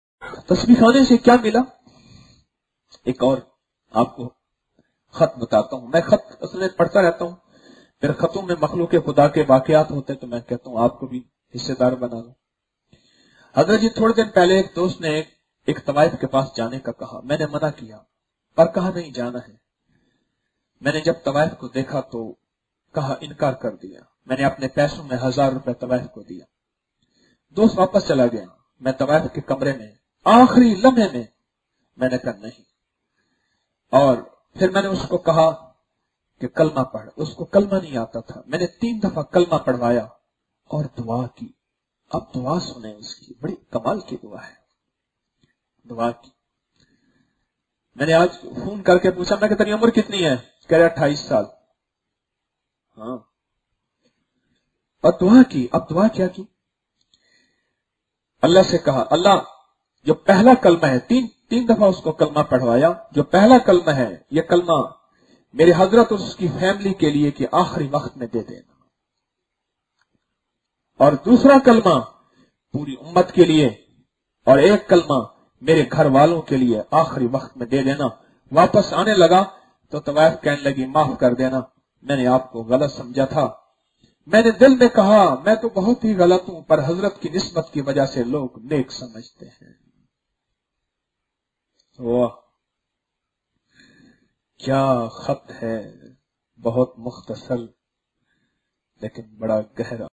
Tasbeeh Khana Sa Kya Mila Hakeem Tariq Mehmood
تسبیح خانے سے کیاملا؟
تسیبح خانے سے کیاملا۔۔۔؟ آپ کوایک خط پڑھ کرسناتا ہوں۔ مخلوق خداکے خط ہوتے ہیں میں سوچتاہوں کہ اس میں بھی آپکو حصے داربنا دوں۔ حضرت جی تھوڑی دیرپہلے ایک دوست نے طوائف کے پاس جانے کاکہا ،میں نے منع کیا پراُس نے کہا جاناہے۔ میں نے جب طوائف کودیکھا توانکارکردیا۔میں نے اپنے پیسوں میں سے ہزارروپے طوائف کودیا ۔ میں طوائف کے کمرے میں چلاگیا۔ آخری لمحے میں میں نے کہا ۔۔۔نہیں۔
اورپھرمیں طوائف سے کہاکہ کلمہ پڑھا۔ اُس کوکلمہ نہیں آتاتھا میں نے اُس کو تین دفعہ کلمہ پڑھوایا اوردُعاکی ۔ اب اُس کی دعاسنیں بڑی کمال کی دُعاہے۔ یااللہ جوپہلاکلمہ ہے میرے حضرت اوراسکی فیملی کیلئے انہیں آخری وقت میں دے دے۔ اوردوسرا کلمہ پوری امت کے لیے اورتیسراکلمہ میرے گھروالوں کے لیے آخری وقت میں دے دینا۔ واپس آنے لگاتوطوائف کہنے لگی کہ معاف کردینا میں نے آپ کو غلط سمجھا تھا ۔میں نے دل ہی میں کہاکہ میں توغلط ہوں مگرحضرت جی کی نسبت کی وجہ سے لوگ نیک سمجھتے ہیں۔